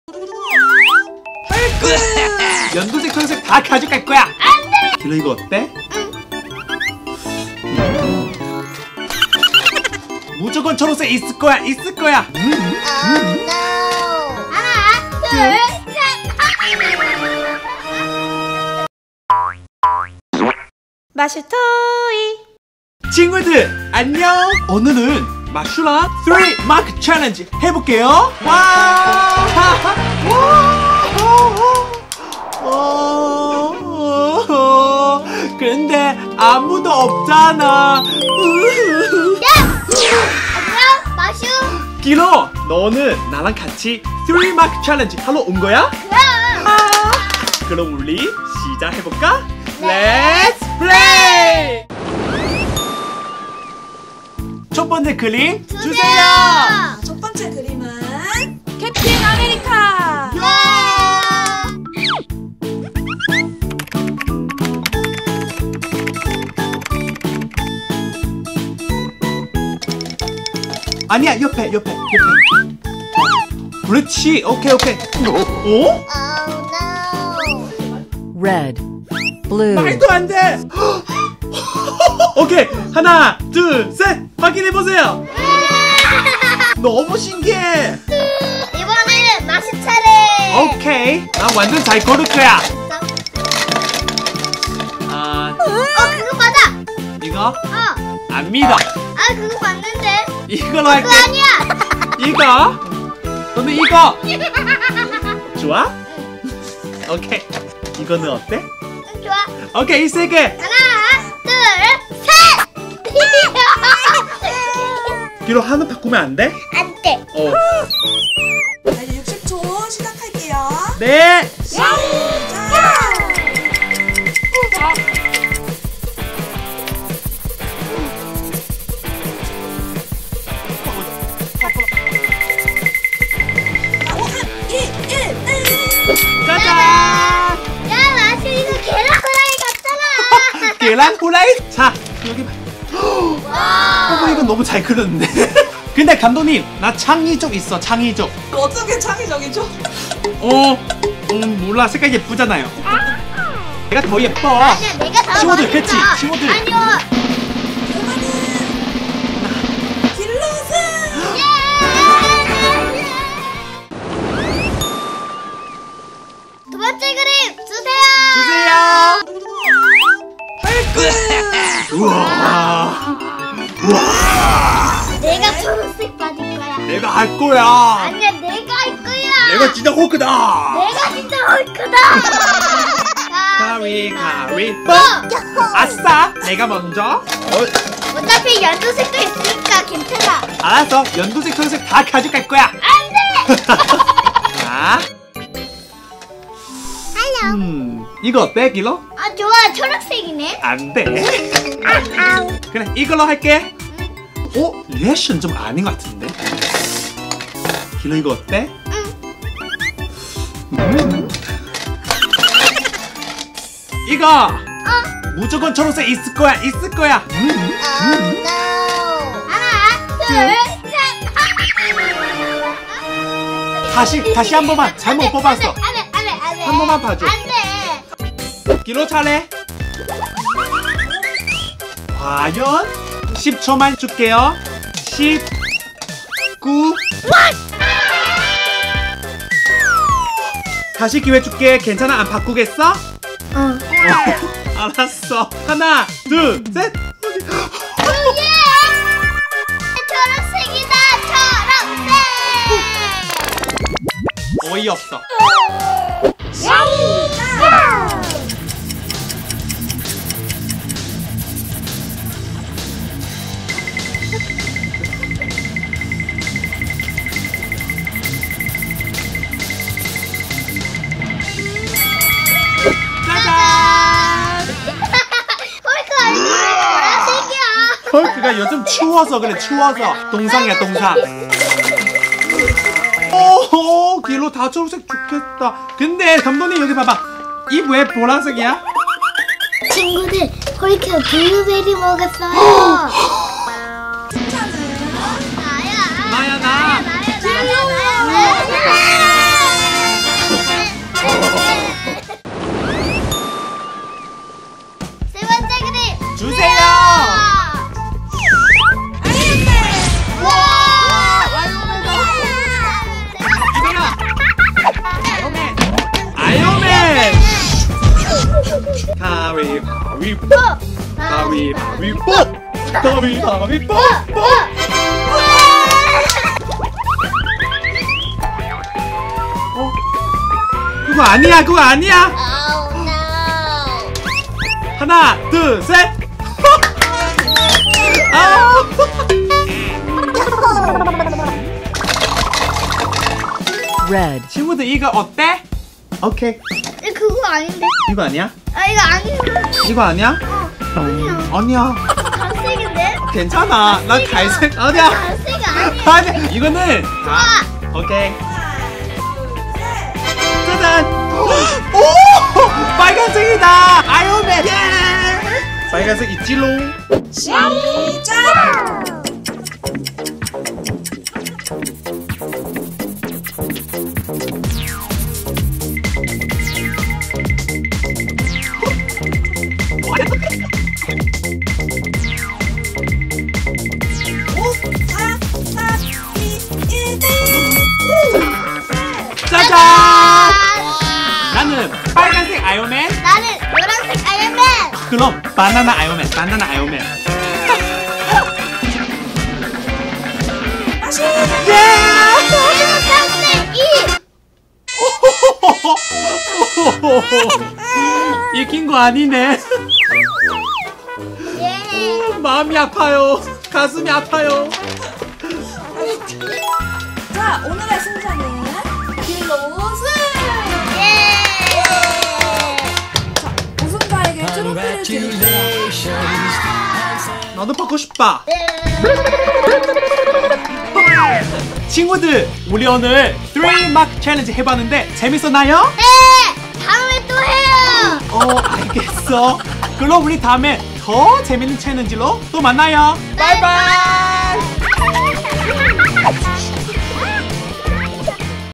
슈퍼히어로 아이언맨 스파이더맨 3가지 색깔로 그리기 챌린지 Superhero 3 Marker Challenge Fun With Spider Man Iron Man - 마슈토이
슈퍼히어로 아이언맨 그림을 같이 그려봐요
Superhero 3 Marker Challenge Fun With Spider Man Iron Man